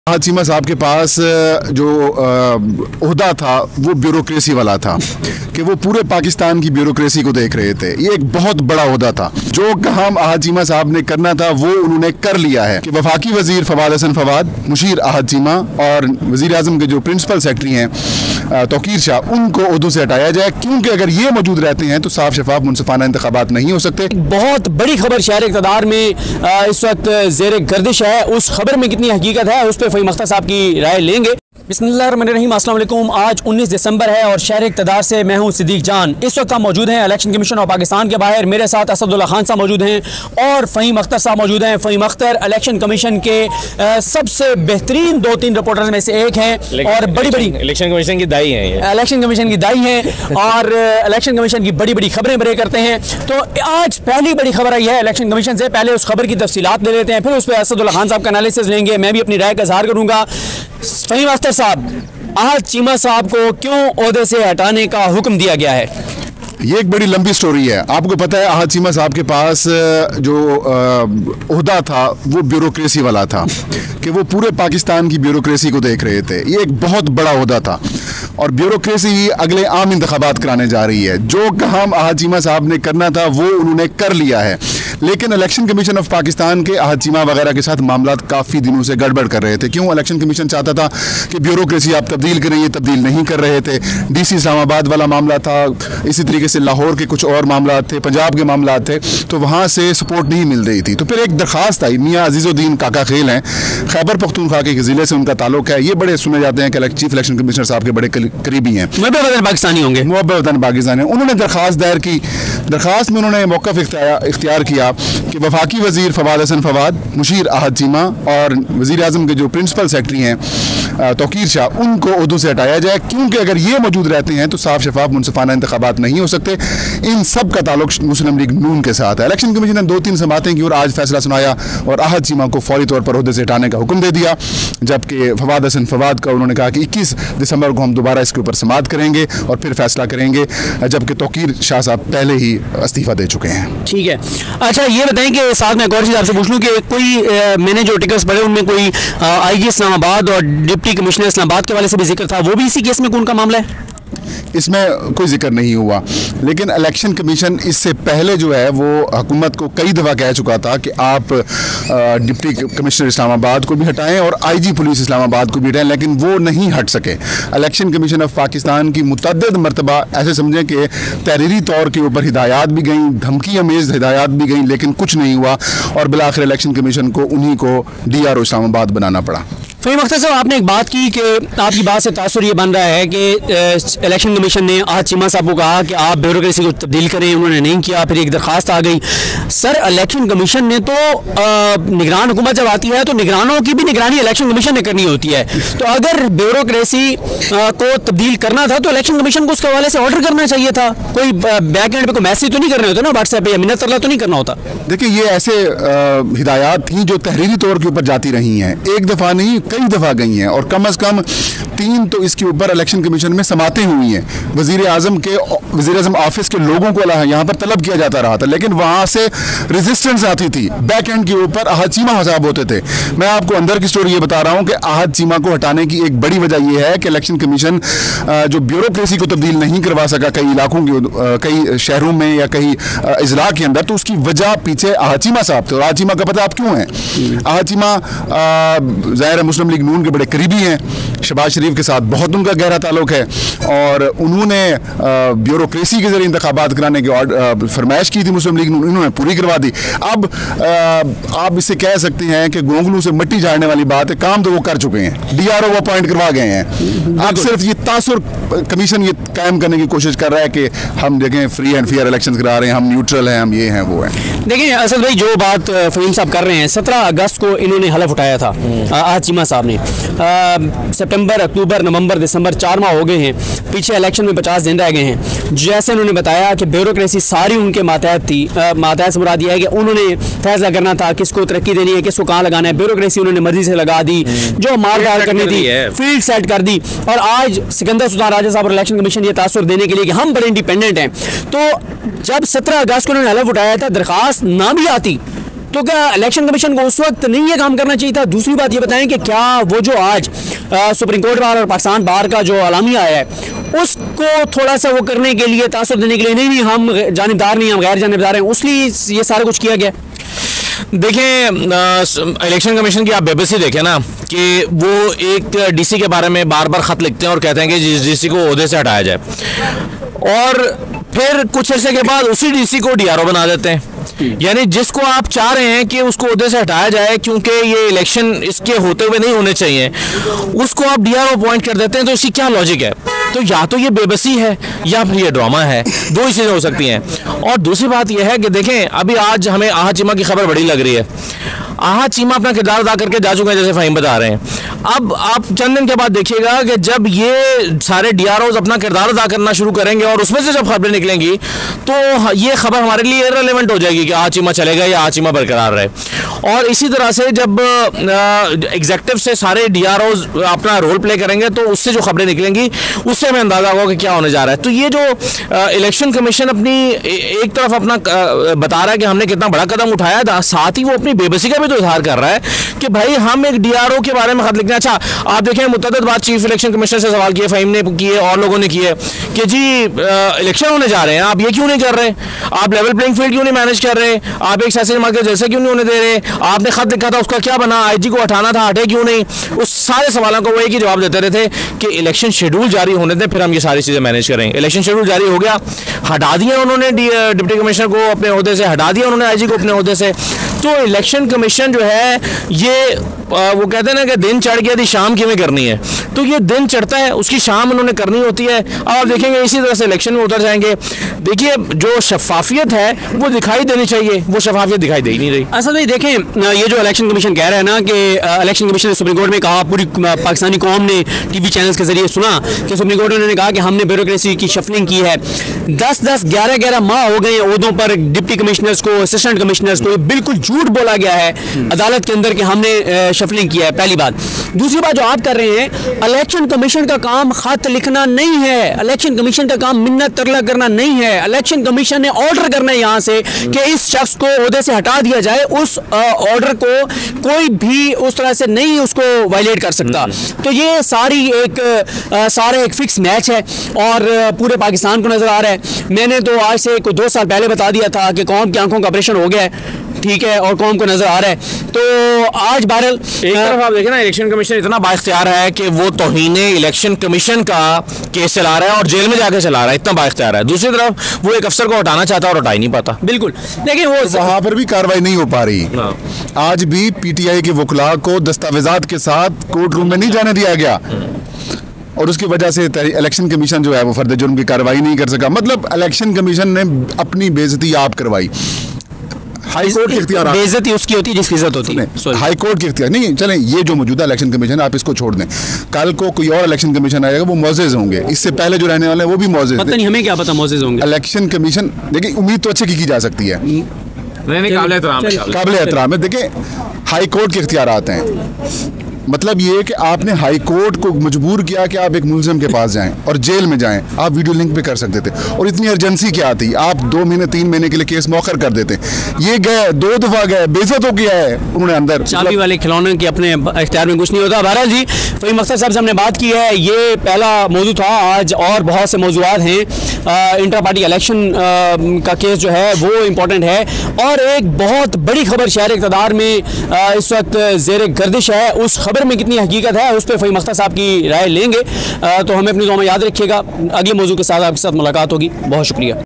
Breaking News || ECP fooled the people for removing Ahad Cheema from office || Siddique Jaan
Siddique Jaan and Asadullah Khan talks about today ECP decision about Ahad Cheema